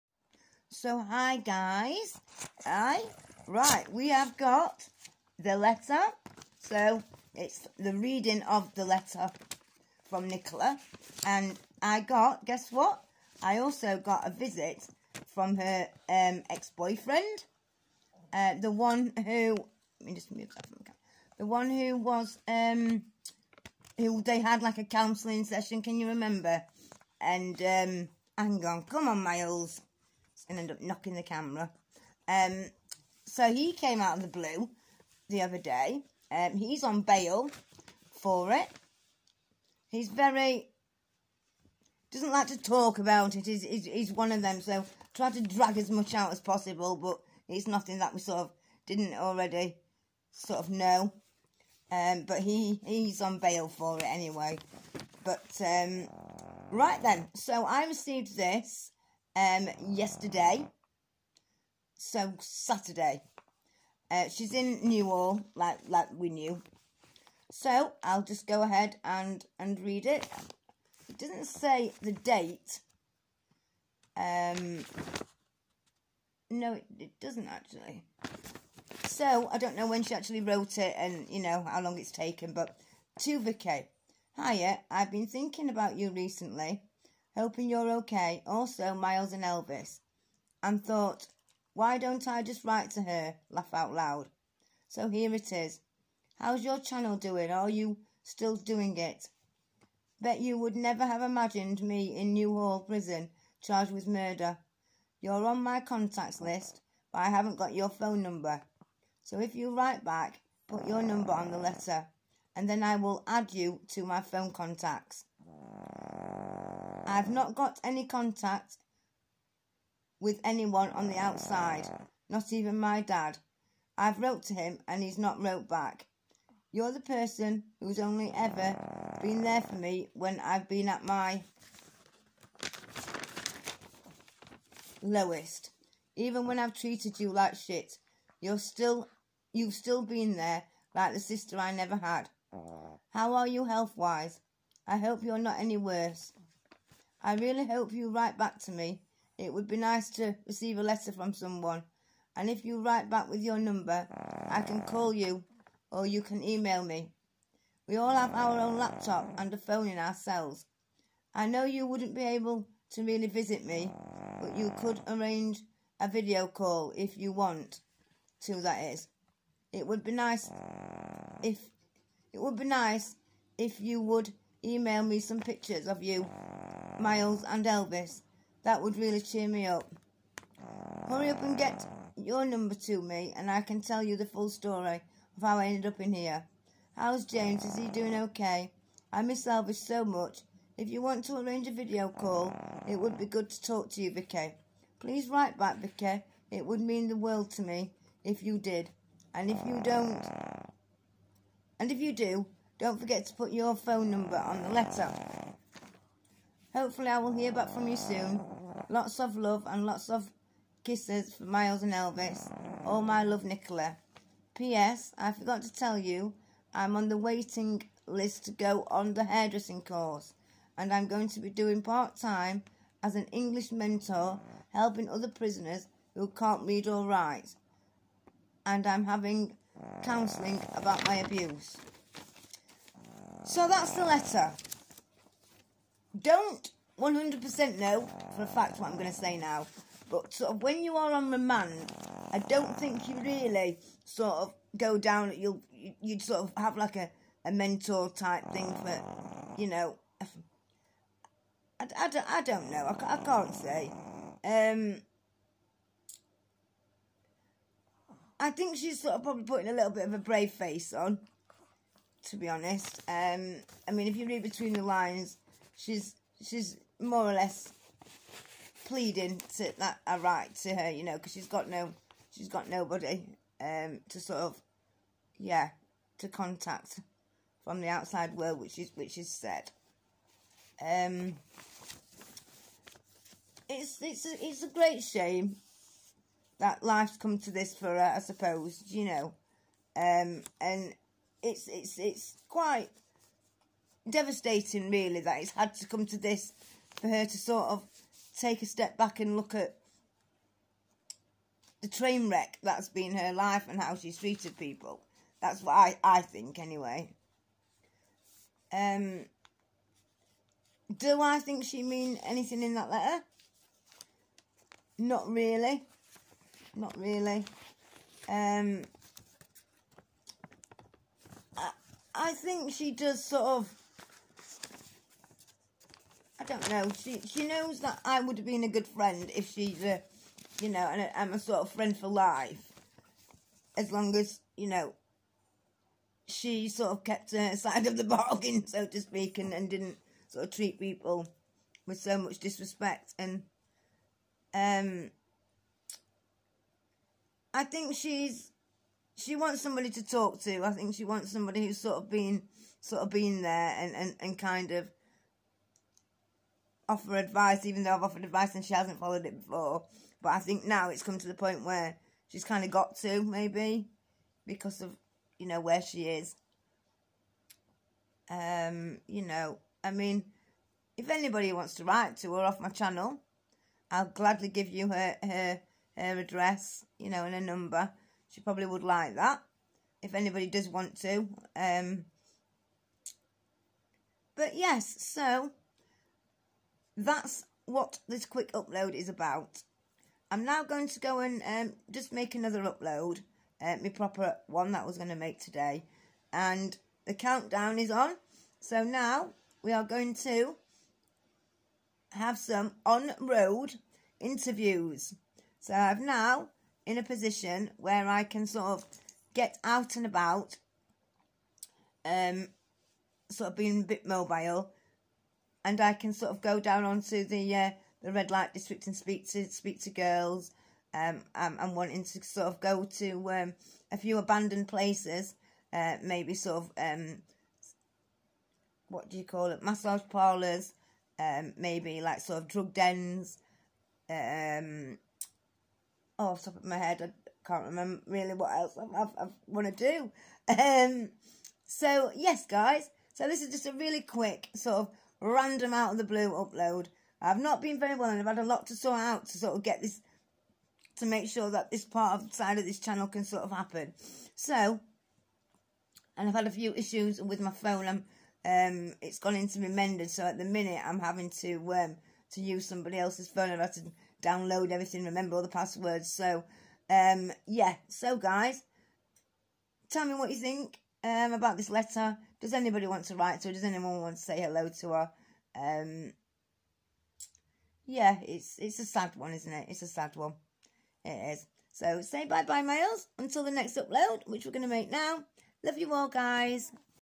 the reading of the first prison letter
Nicolas first prison letter has arrived